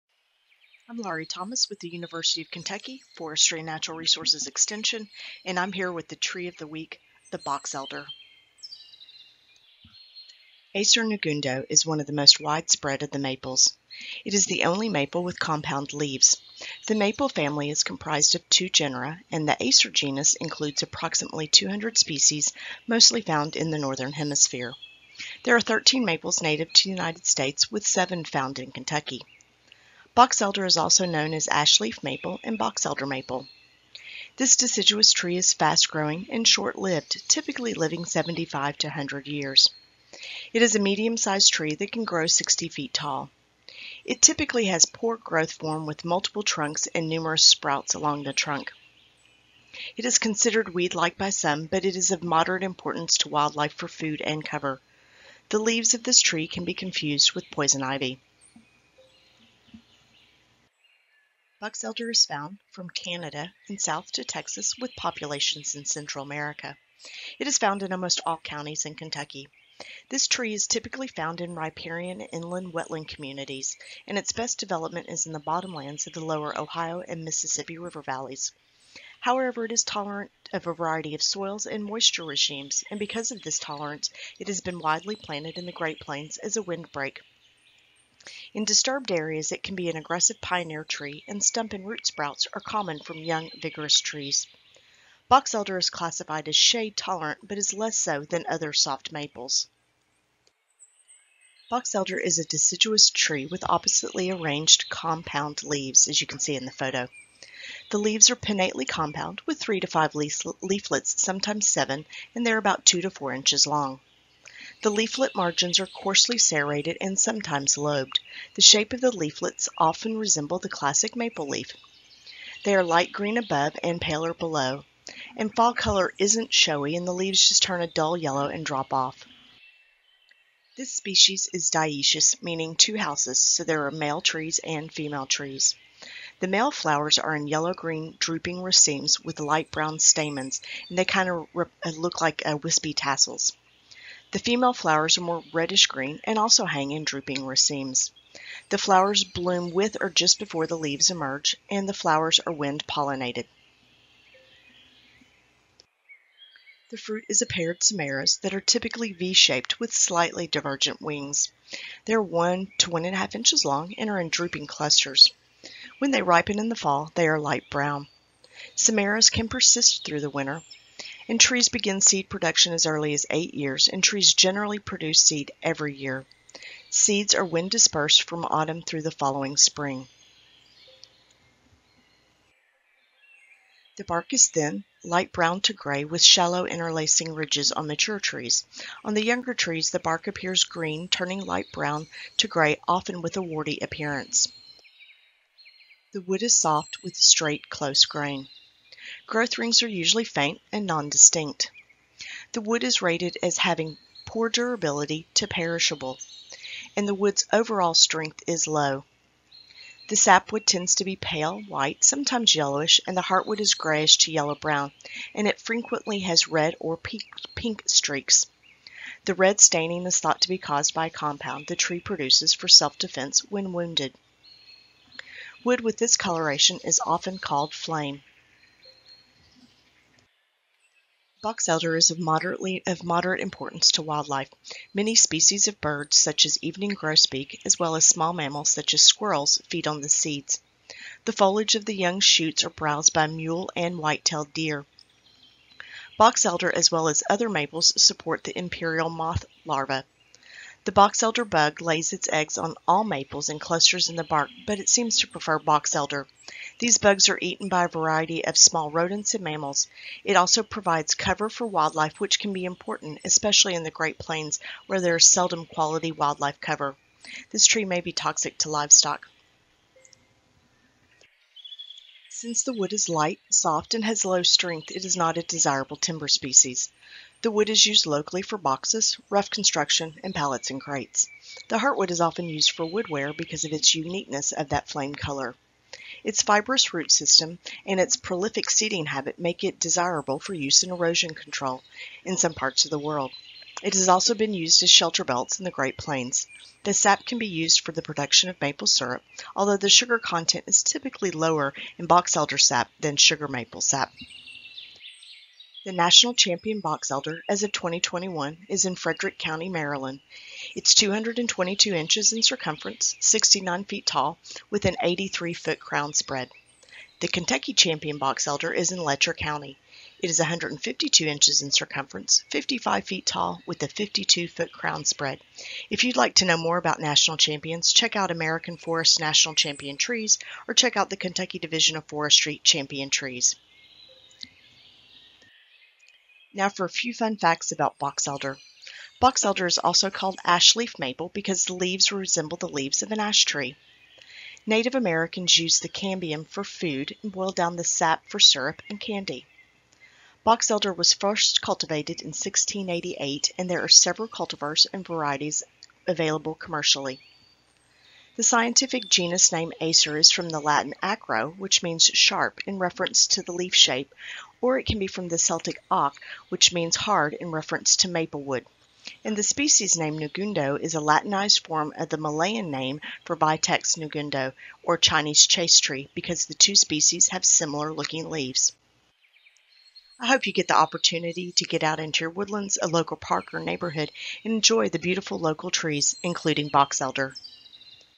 Tree of the Week: Boxelder
Boxelder is one of the maples native to Kentucky and because of its compound leaves, it is sometimes referred to as "ashleaf" maple.